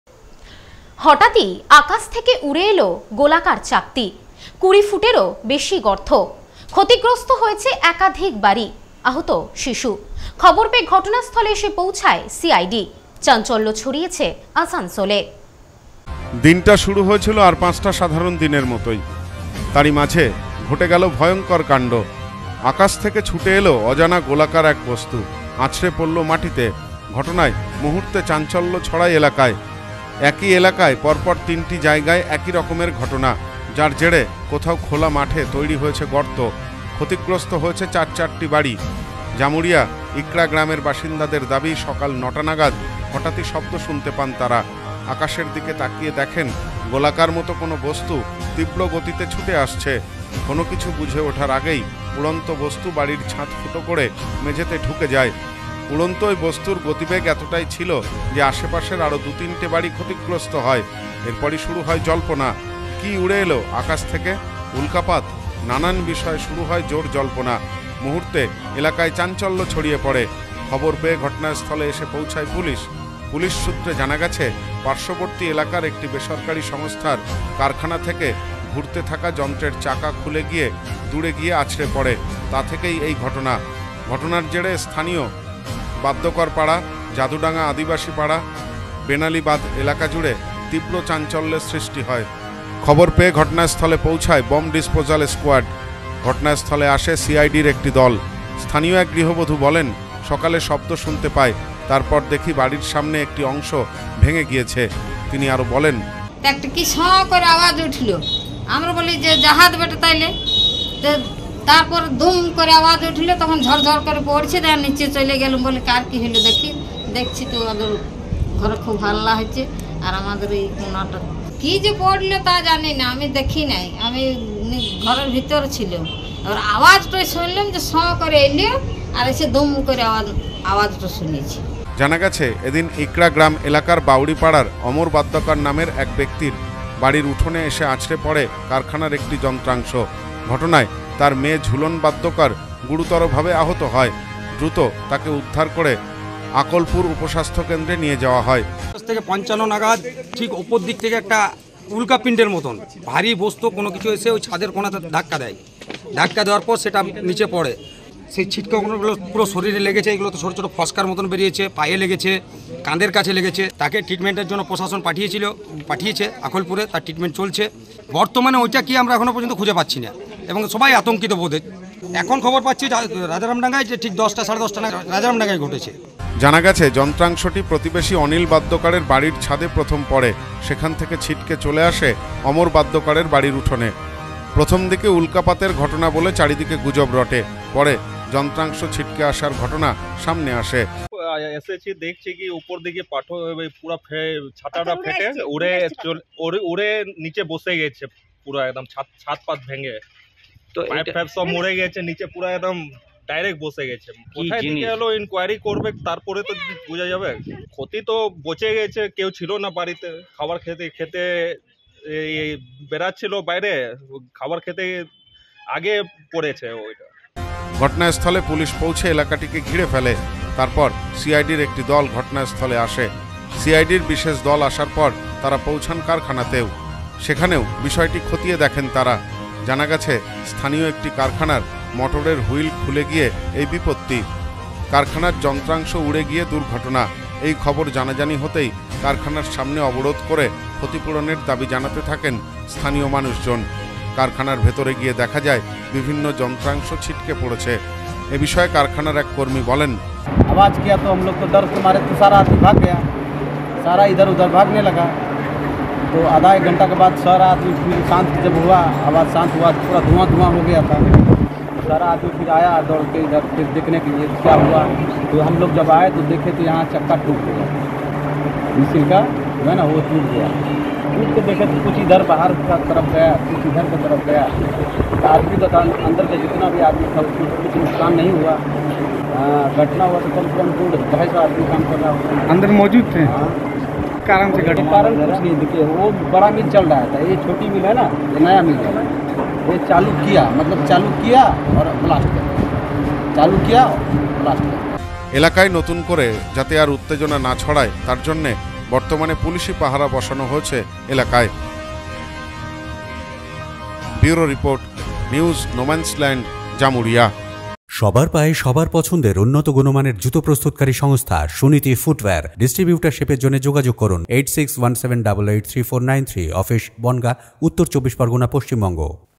হঠাৎ আকাশ থেকে উড়ে এলো গোলাকার চাকতি!
হঠাৎ আকাশ থেকে উড়ে এলো গোলাকার চাকতি! কুড়ি ফুটেরও বেশি গর্ত। ক্ষতিগ্রস্ত একাধিক বাড়ি, আহত শিশু। ঘটনাস্থলে এলো সিআইডি। চাঞ্চল্য আসানসোলে।